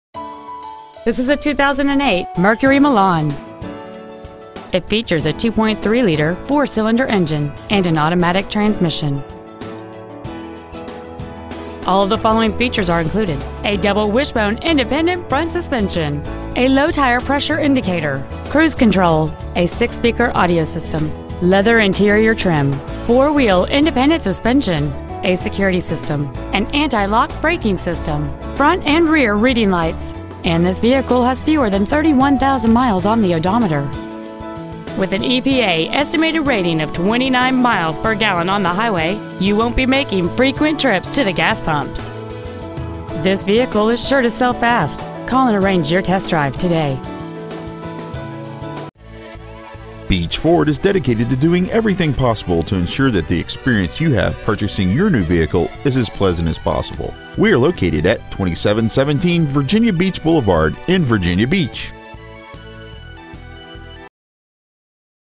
Preowned 2008 MERCURY MILAN Virginia Beach VA
Year : 2008

 Make : MERCURY

 Model : MILAN

 Engine : 2.3L I4 16V MPFI DOHC

 Trans . : AUTOMATIC

 Exterior : WHITE SUEDE

 Miles : 30,664

 2717 Virginia Beach Blvd